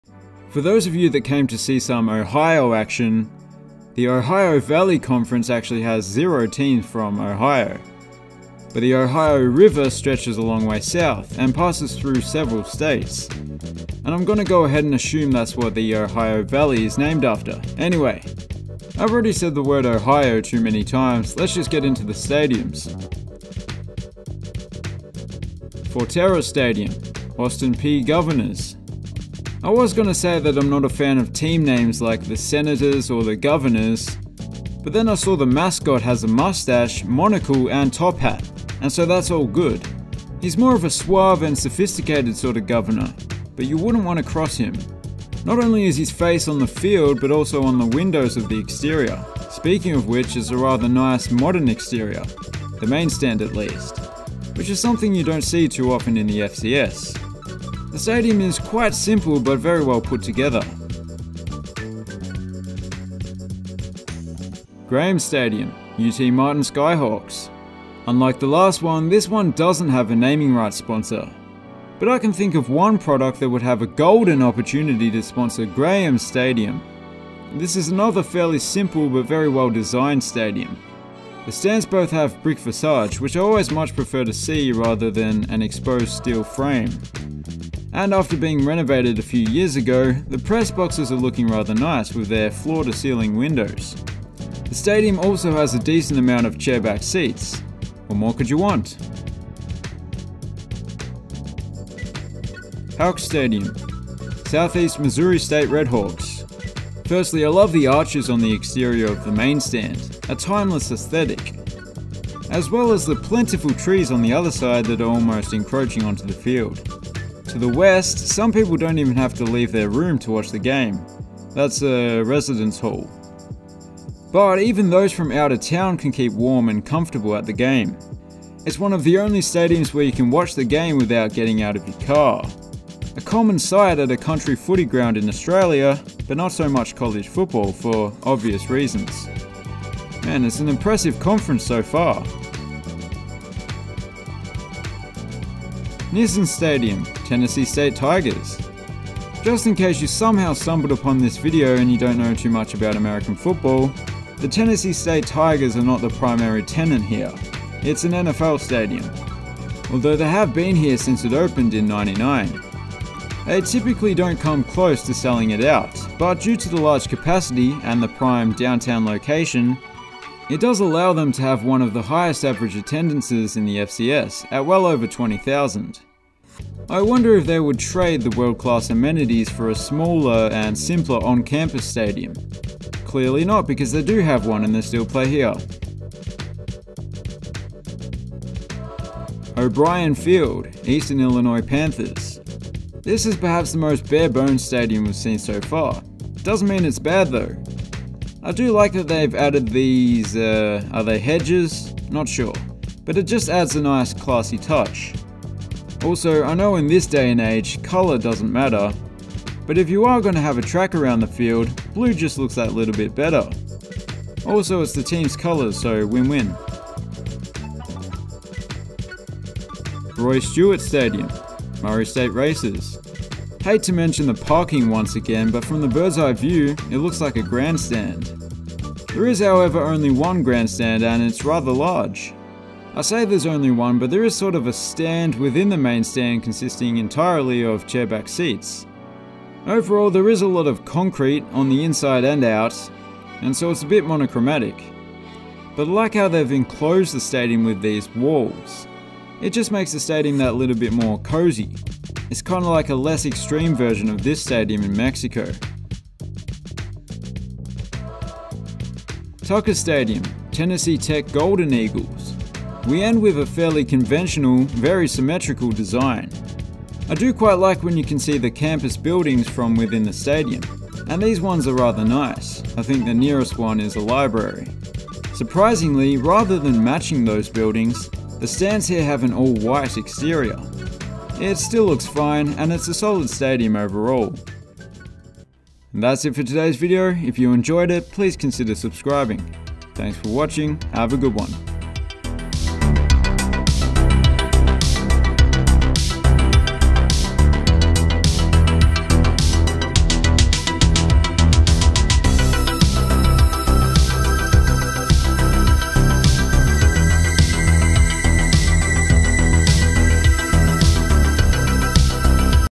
Ohio Valley College Football Stadiums
We showcase the stadiums of the Ohio Valley Conference. One of the many NCAA Division I FCS conferences.